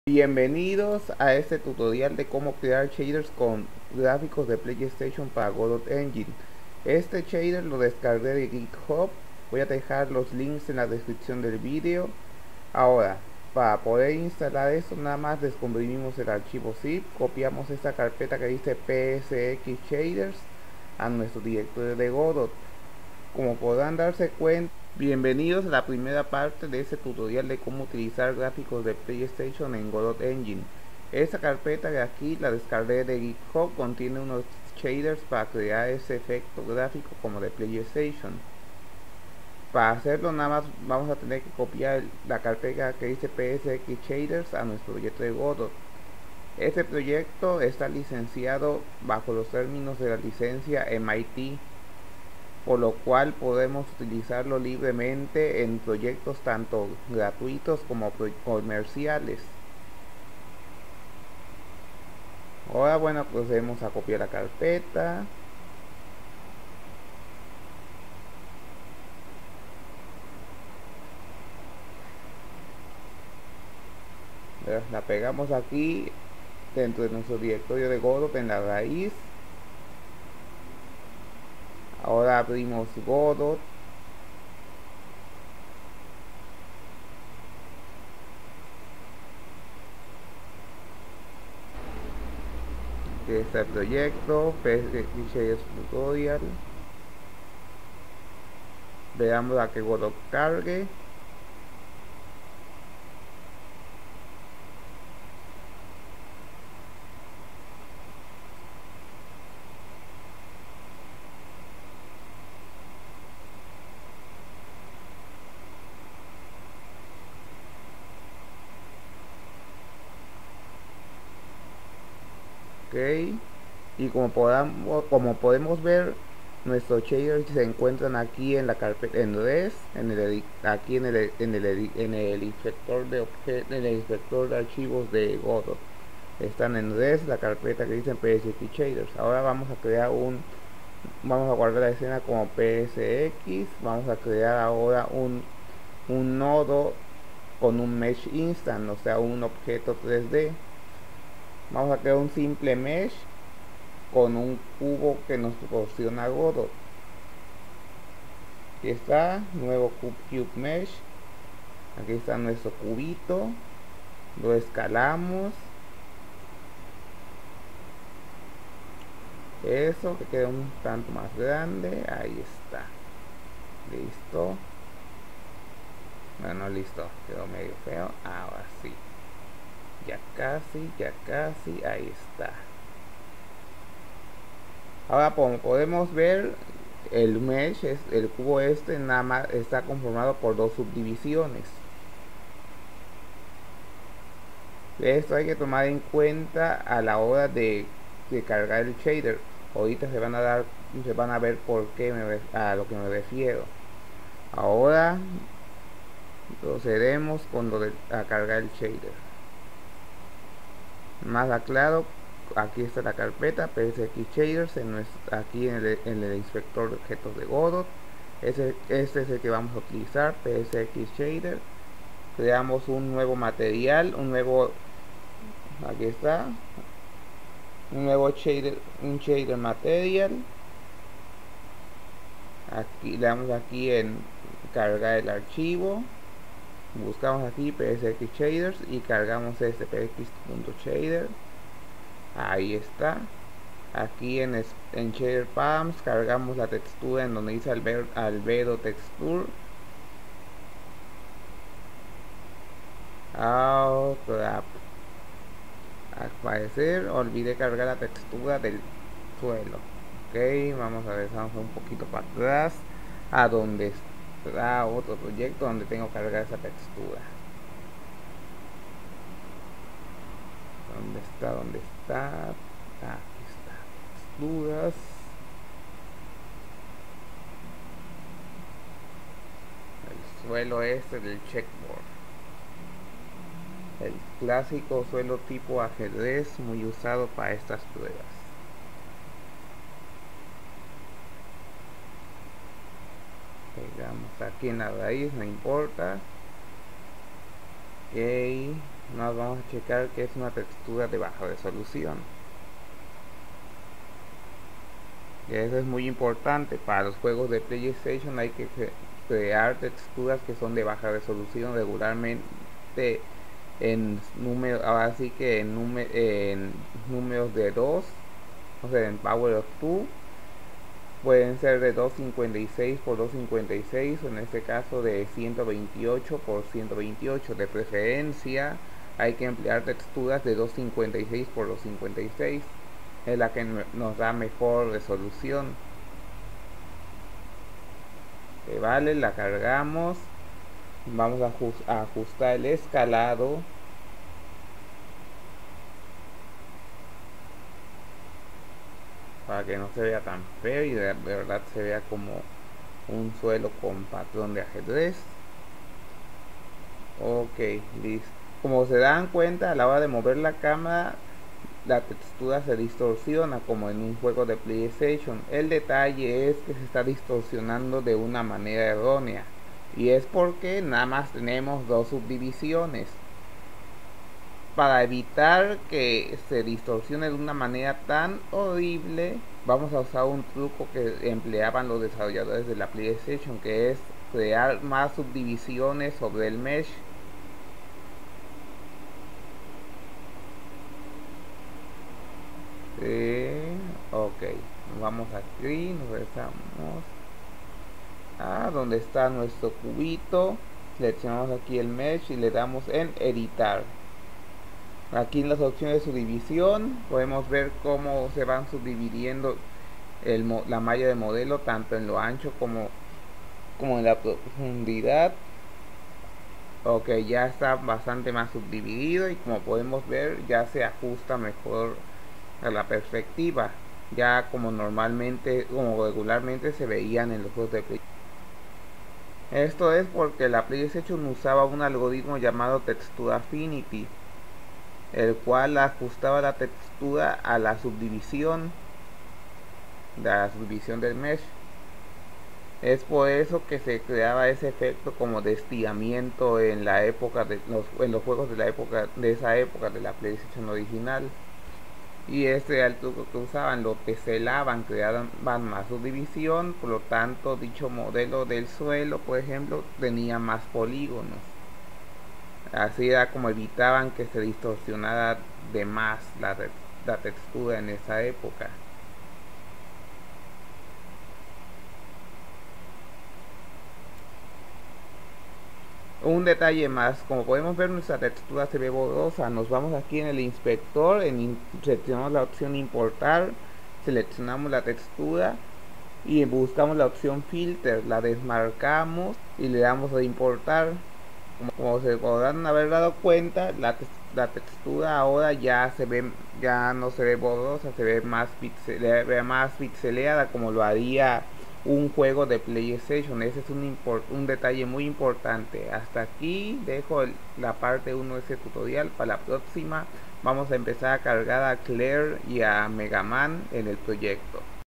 Tutorial PSX Shader Godot Engine Parte1
Parte uno de una pequeña serie de vídeos de como utilizar Shaders para crear juegos con gráficos tipo PlayStation 1 (PSX).
Dejo un link al repositorio en Github donde puedes descargar los Shaders.
Hasta pronto.